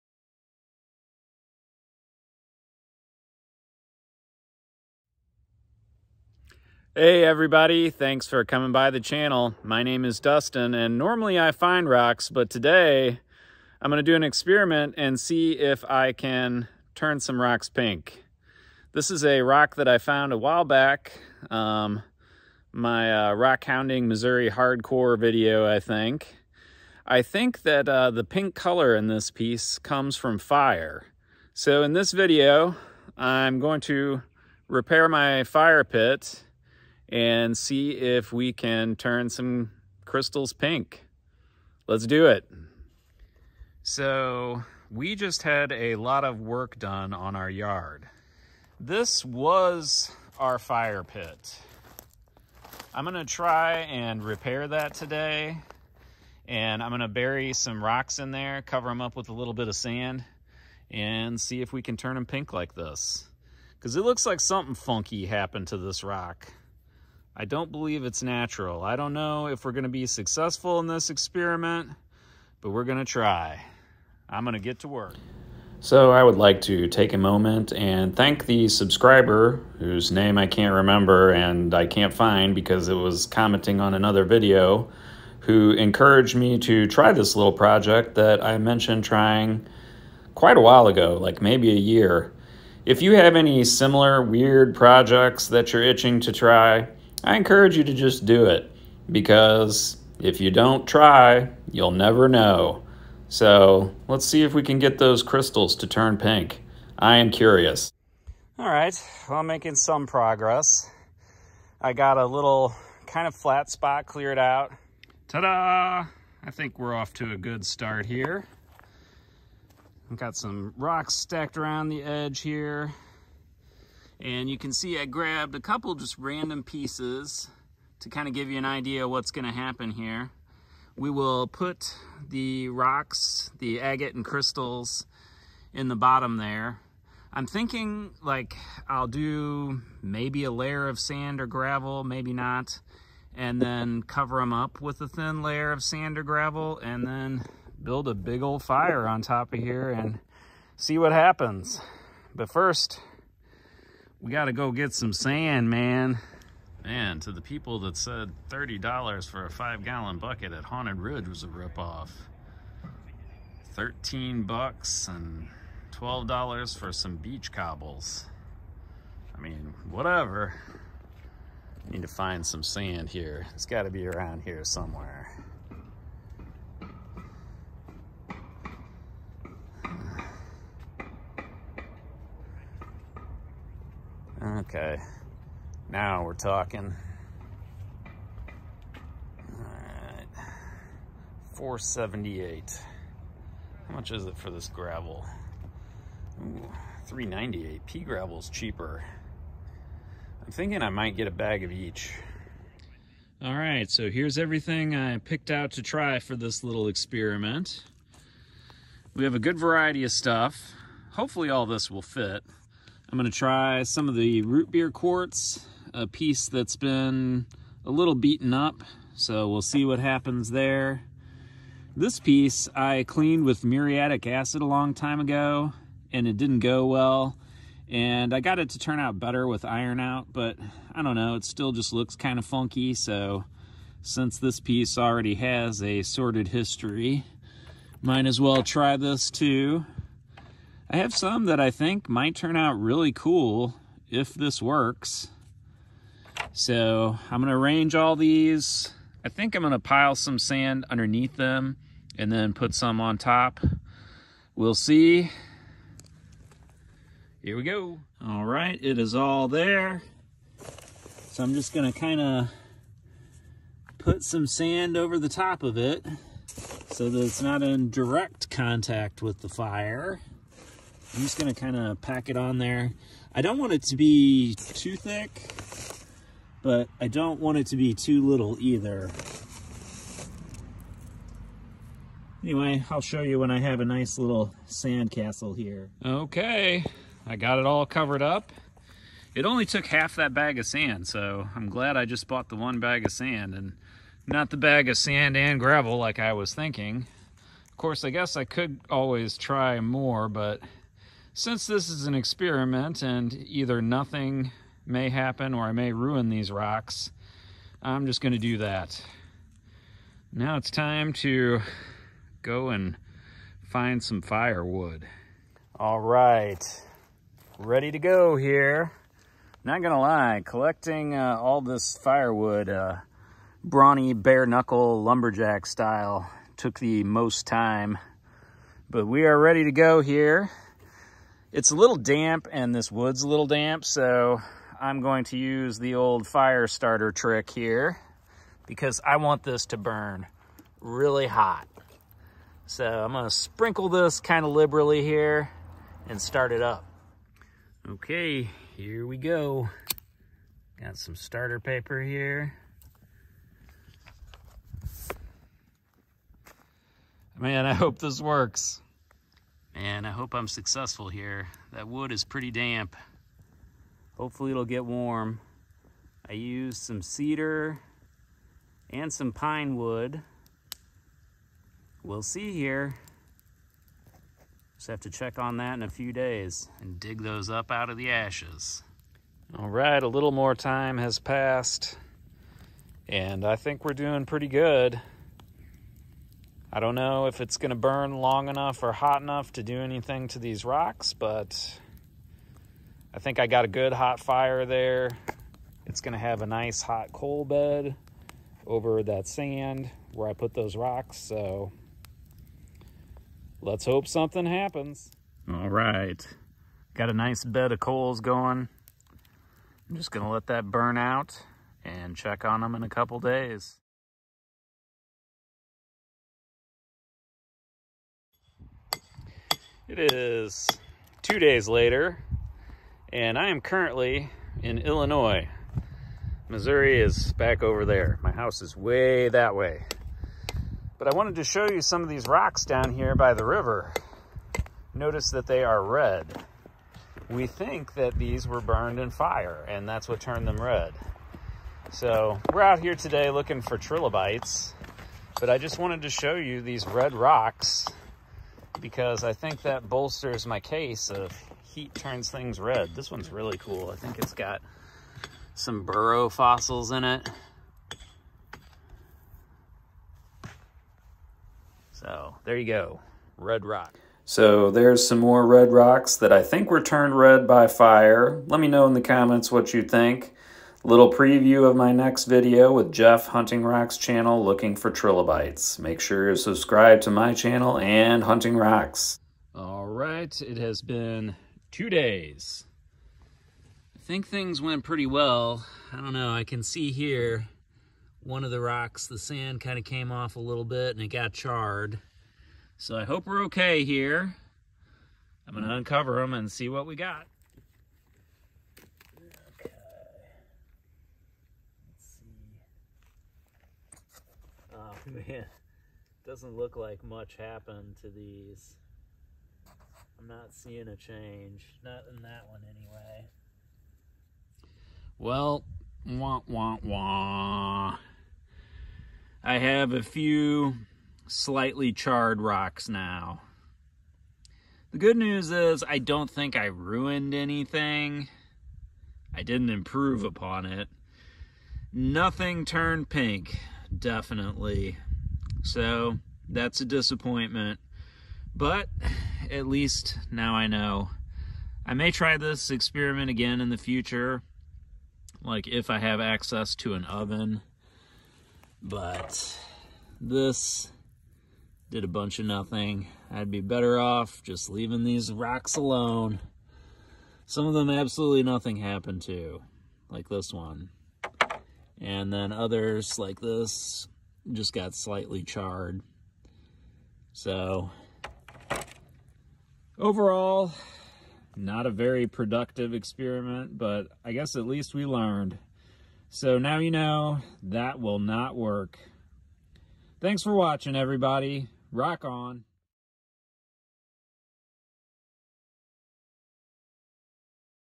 Heat treating Missouri quartz / Will it turn pink?!
In this video I experimented with heat treating Missouri lace agate druse using a fire.  Will it work? Let’s find out!